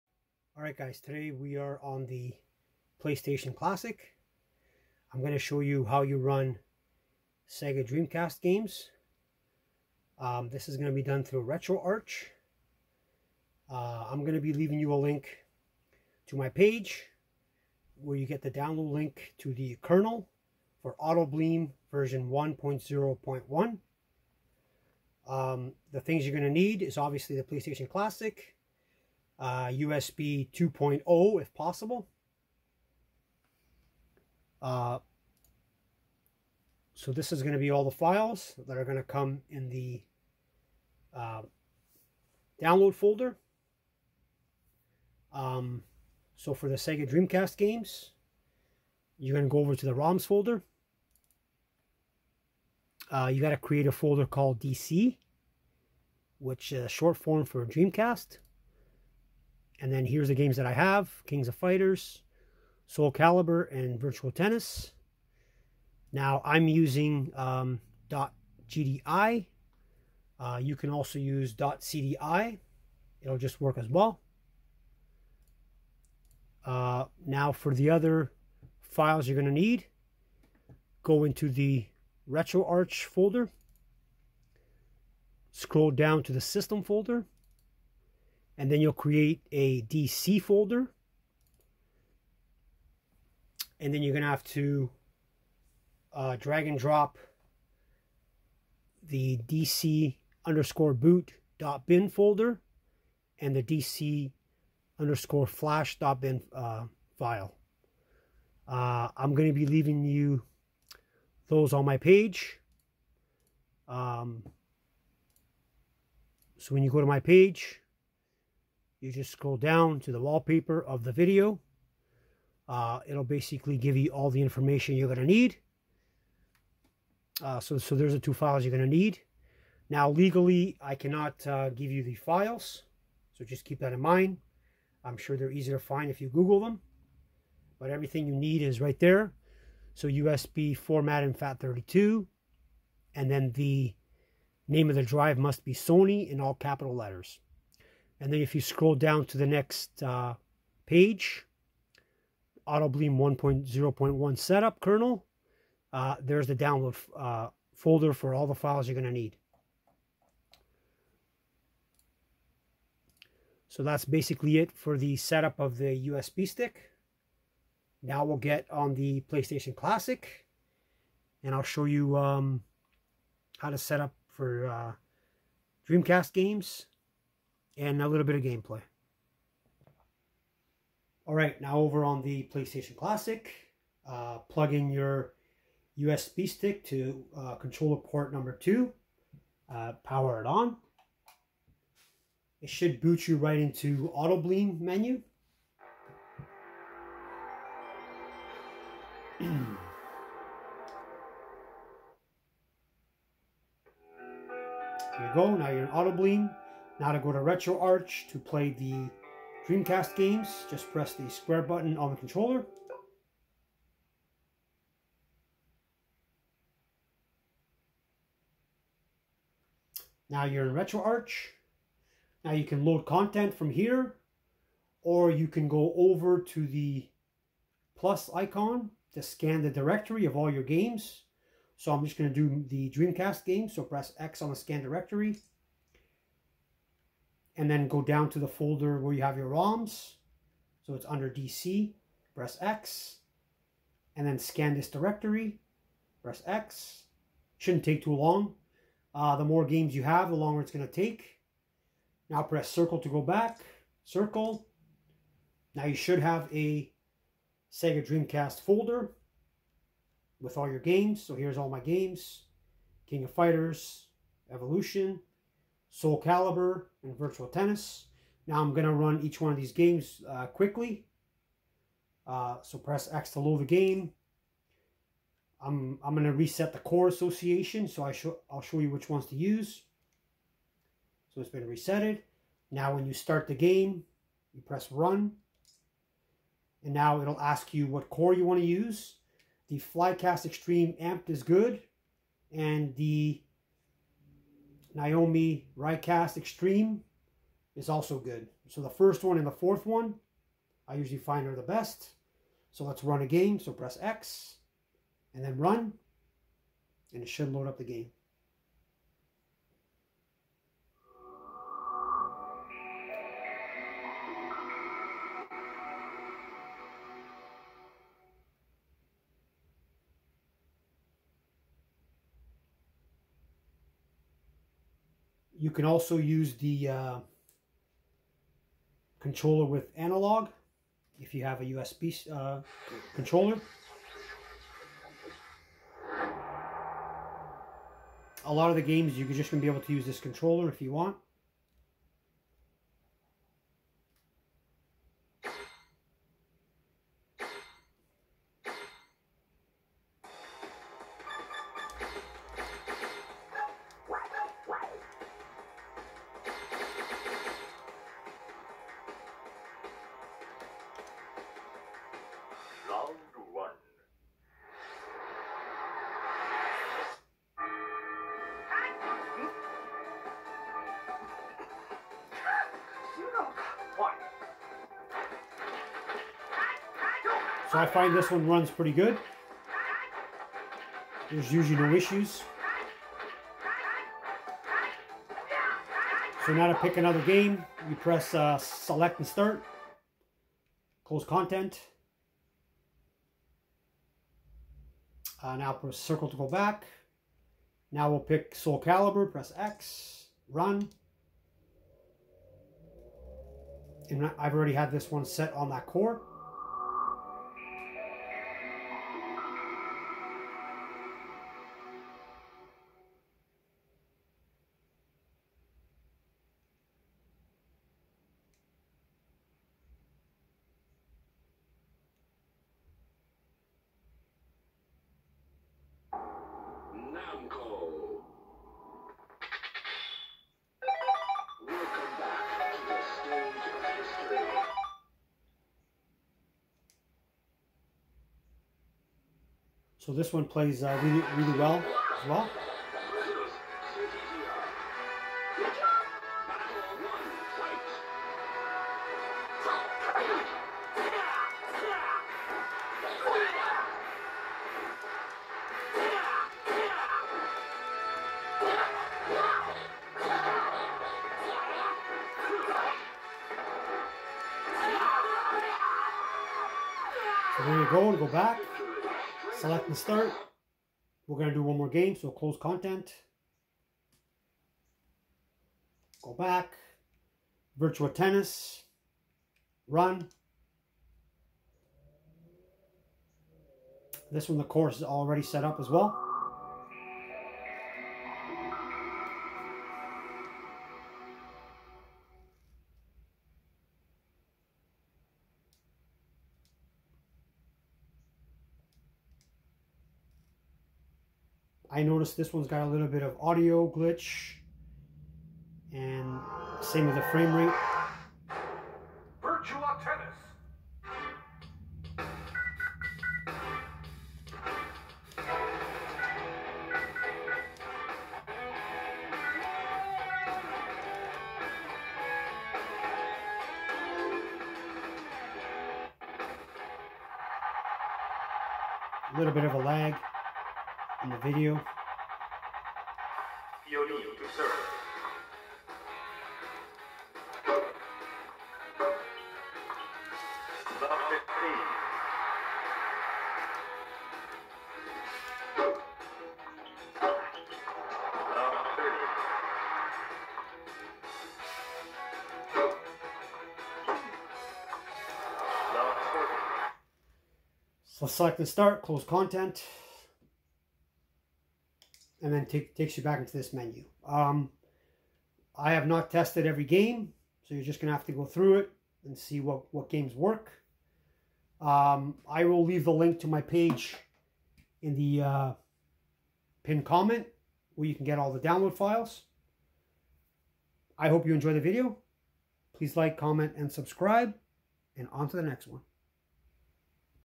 How to setup Sega Dreamcast games on the playstation classsic!
* Please check out my previous video to get the kernel 1.0.1 download!
* Make sure you copy all the folders to the root of the SD card Formatted in Fat32 & name the drive SONY in all capitals!
Create a DC folder in Retroarch for your roms & Create a DC folder in the System menu in Retroarch to add your bios files =
dc_boot.bin & dc_flash.bin